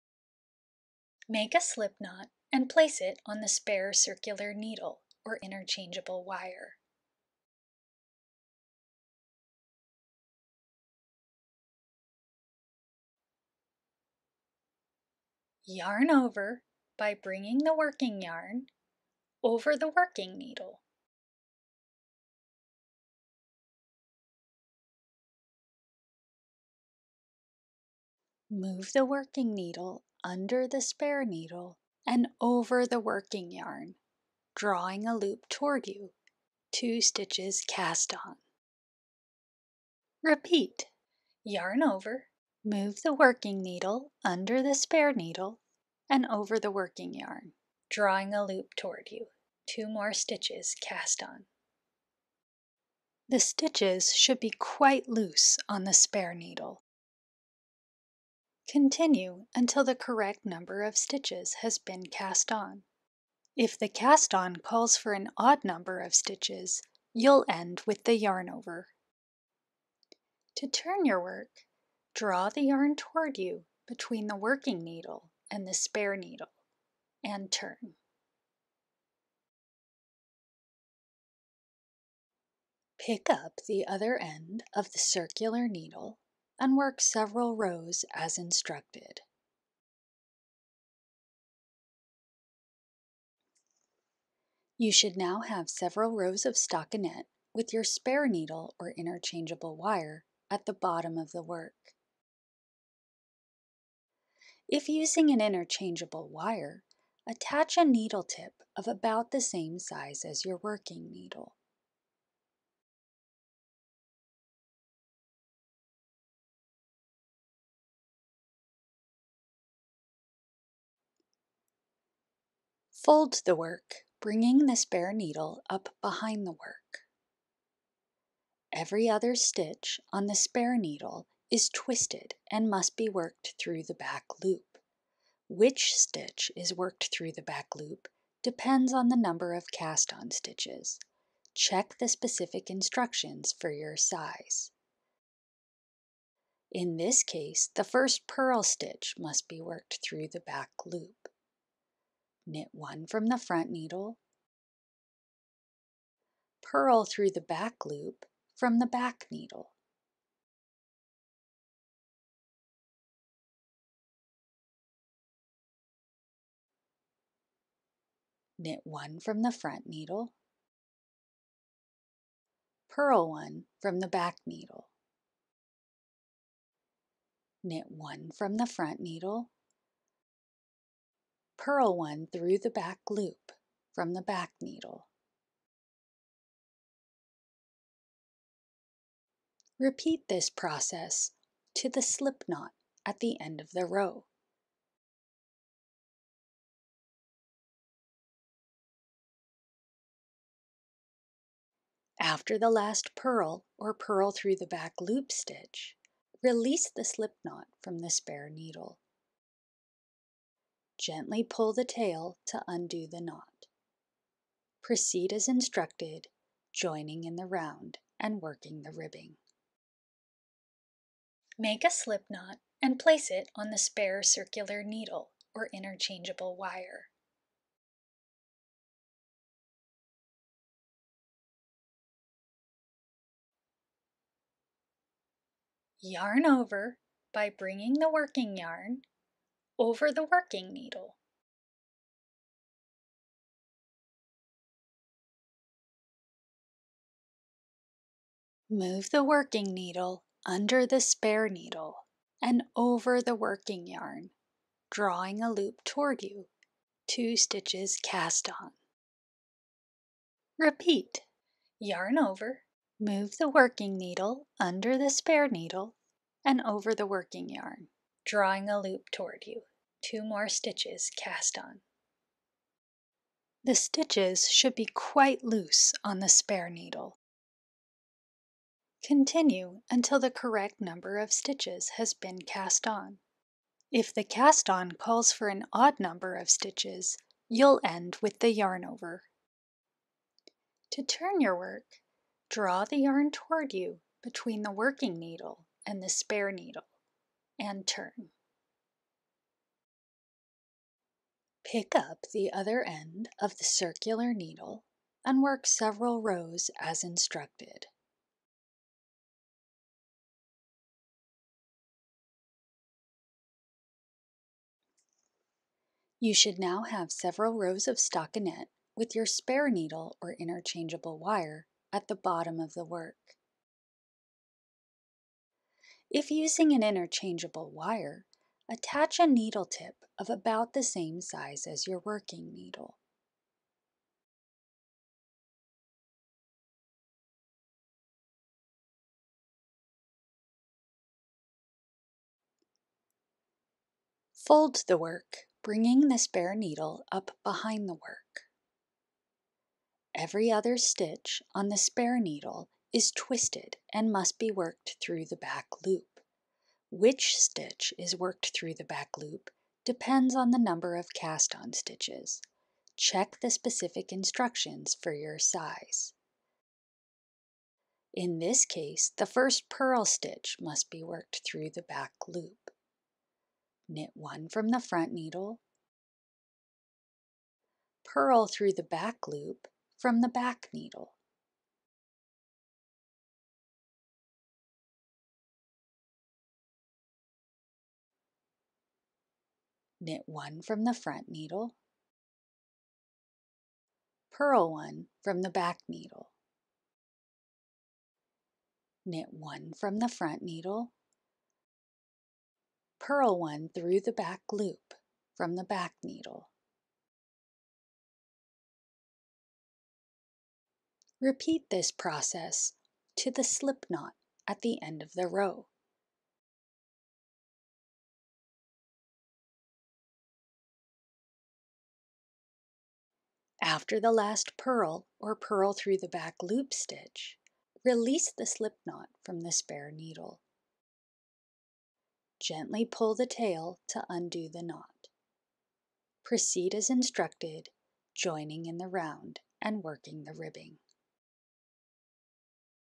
Tubular Cast On with Spare Needle Method (RH & LH)
Right Handed: 0:00
Left Handed: 3:38

This video shows the full tubular cast on for 1x1 ribbing using the Spare Needle Method. Provisionally cast onto a spare needle or interchangeable wire, work a few rows of stockinette, then combine this stitches by alternatingly working off each needle. This creates a clean folded edge perfect for necklines. We recommend practicing a few times to make sure your cast on is loose enough to fit over your head or around your waist. This cast on can be aggressively blocked to widen.